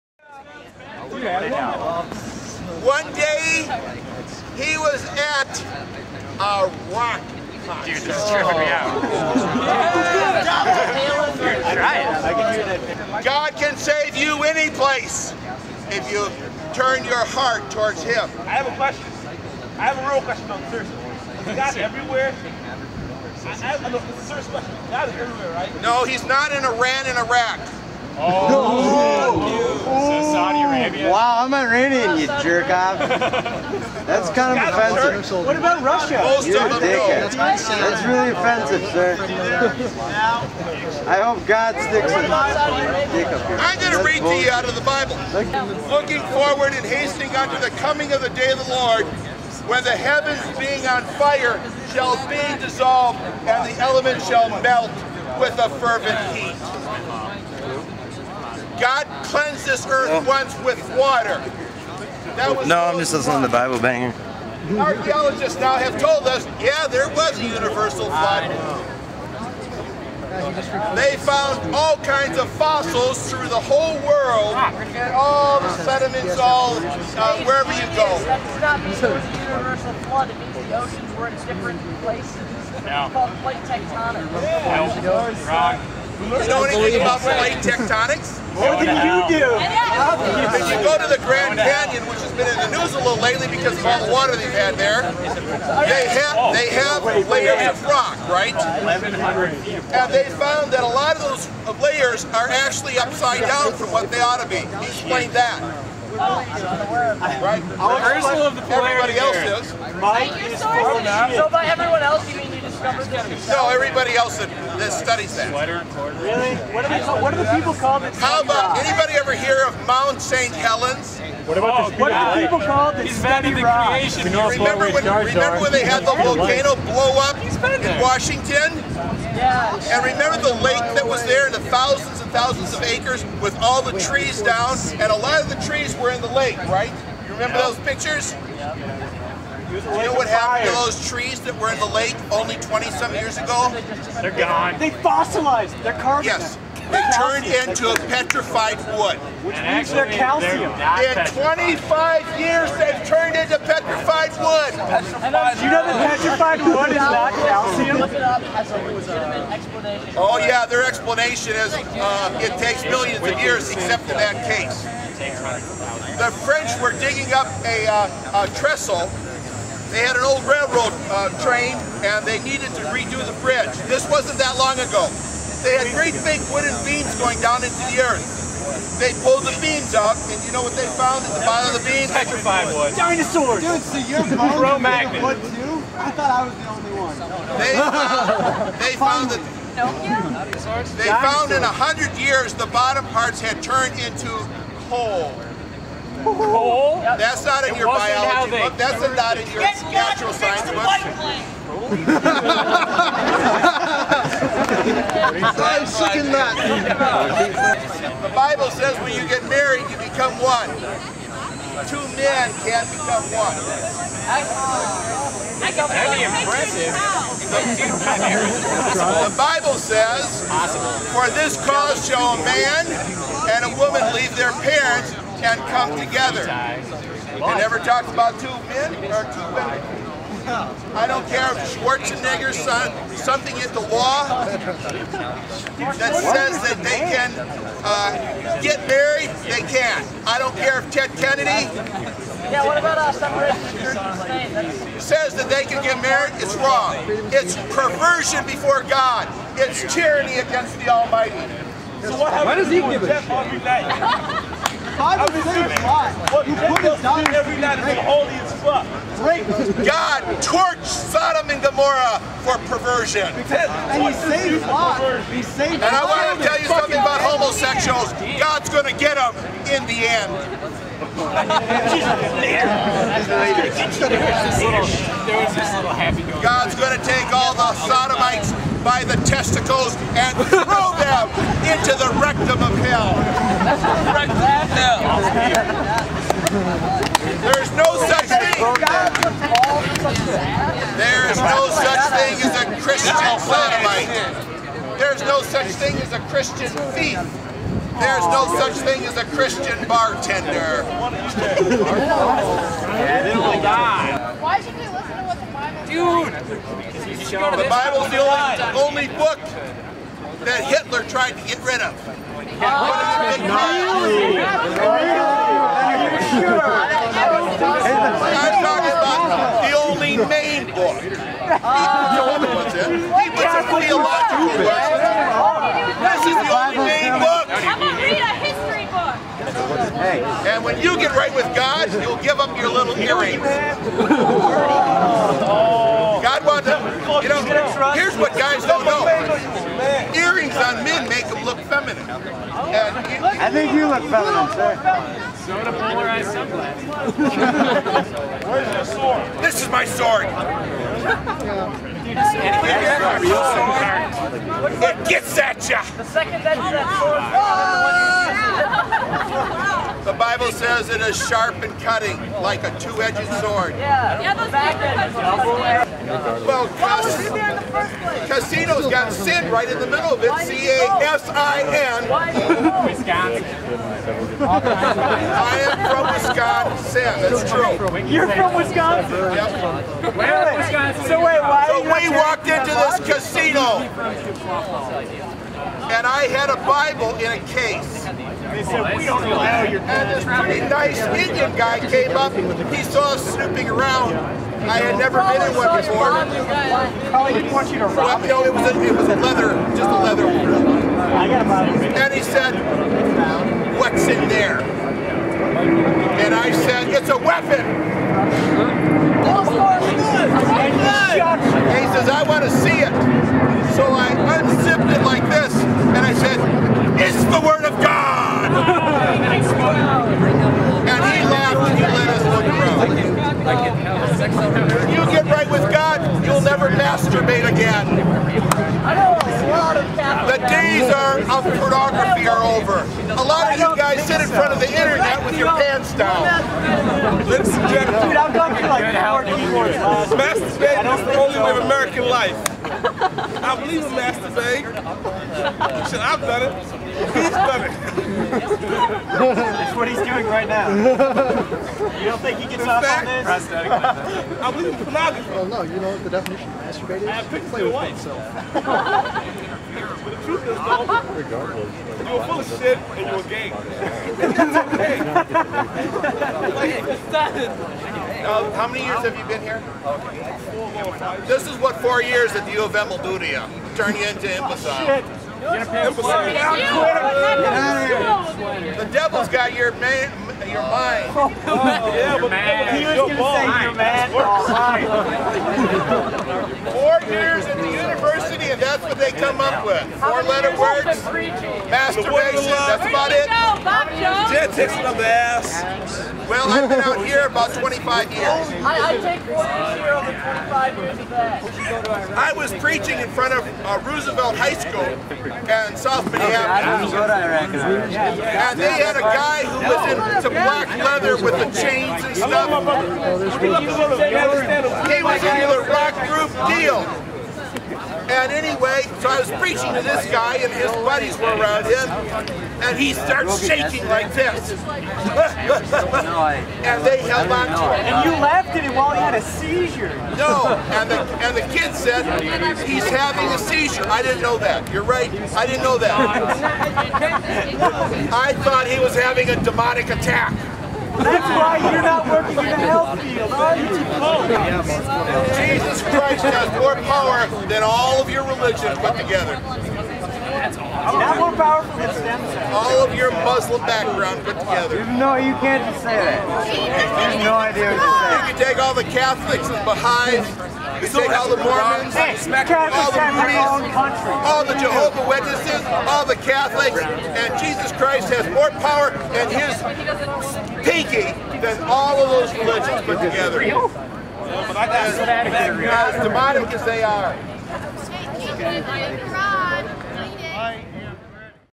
biblethump.wmv
"Just as God destroyed Sodom and Gomorrah, the Twin Cities, Saint Paul and Minneapolis, are next on God's hit list." -Brother Mike